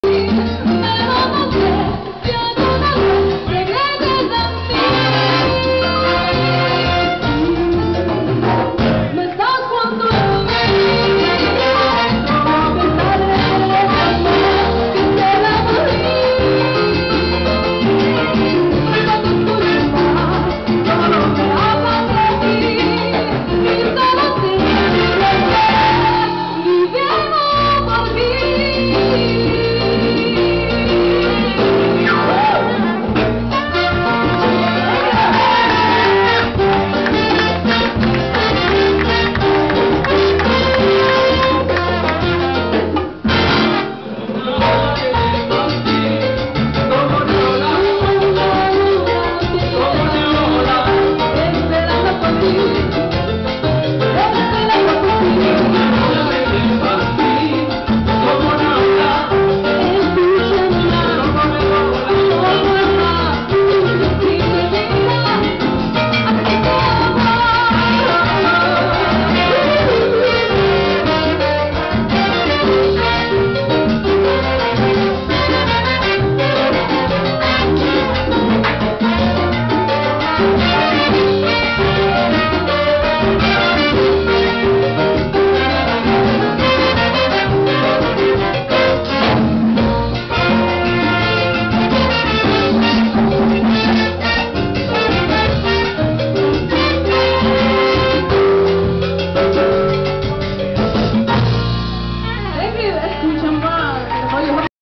Mandarina China Band at Jz Club- Shanghai
Very nice performance at JZclub in Shanghai China directed by Alvaro Cardenas,Jr.